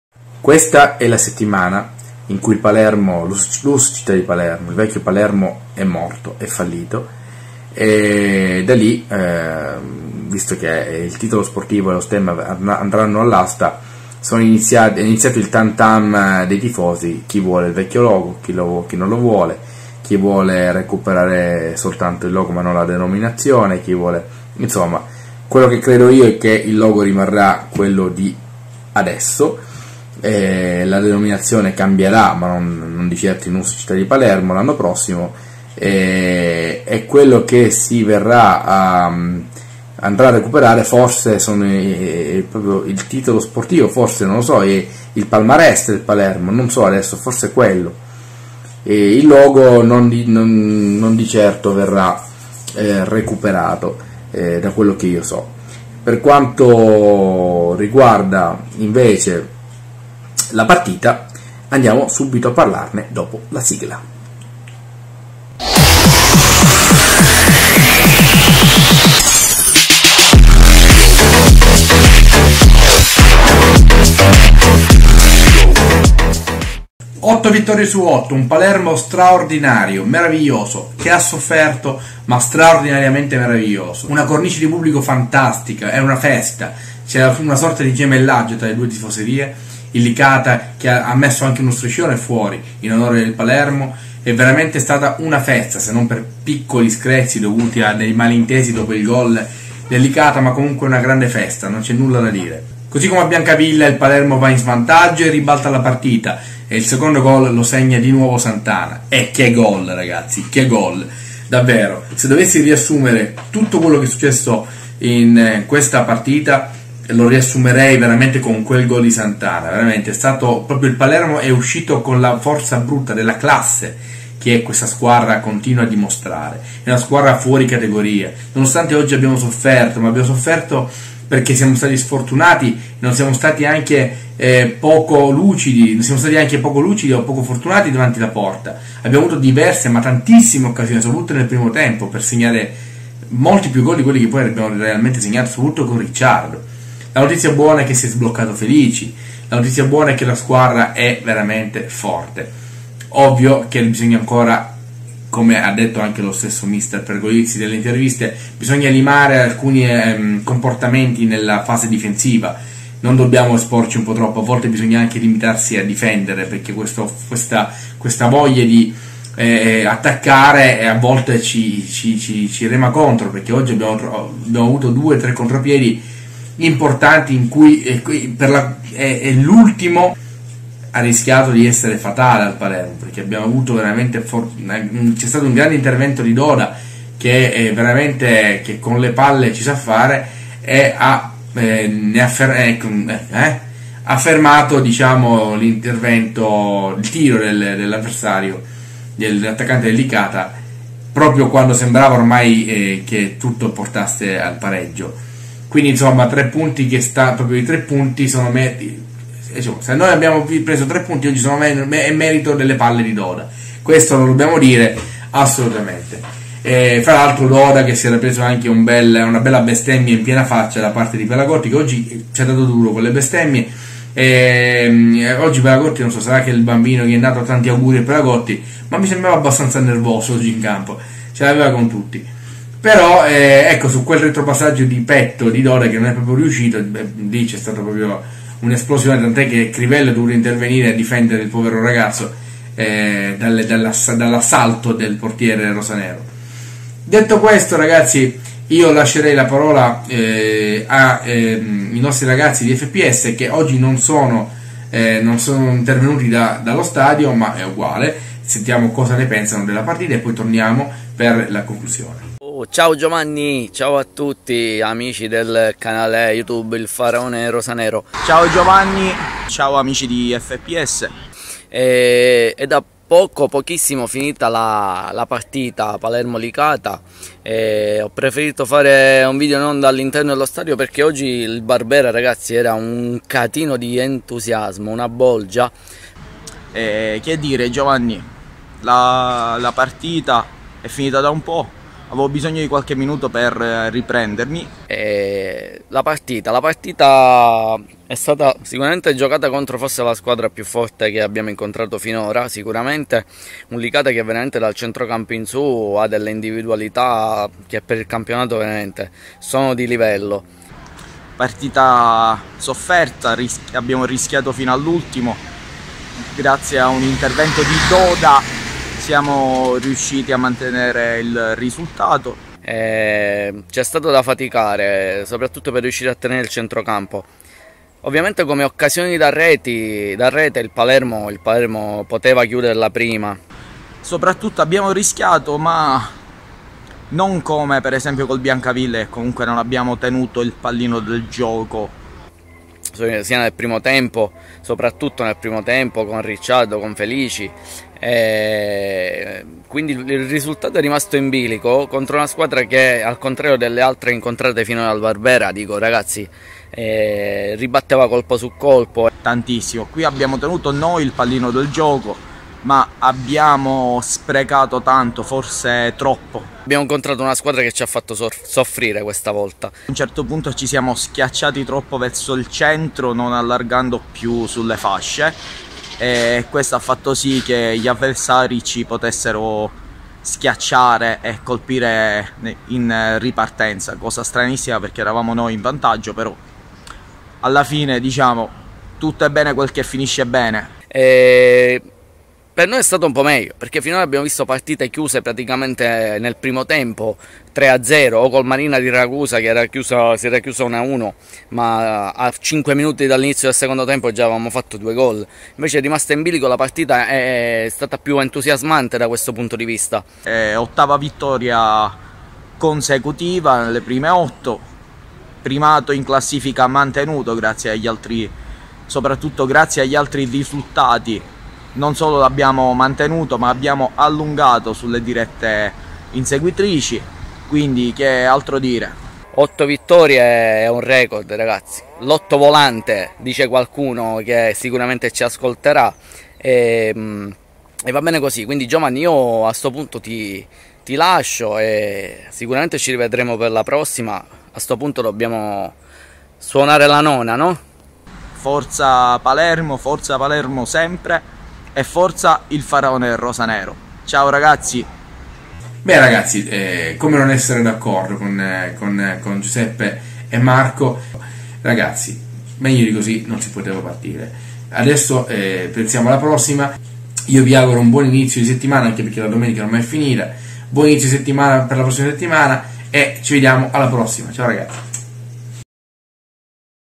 Prova a Prendermi...PALERMO - LICATA 2-1 - Feat. FPS
Il Palermo è inarrestabile, vince ancora e si porta al primo posto, 8 punti sulla seconda, la strada è ancora lunga ma questo Palermo sembra davvero fuori categoria!
Video commento insieme a Giuseppe Fontana e Marco Fontana di FPS!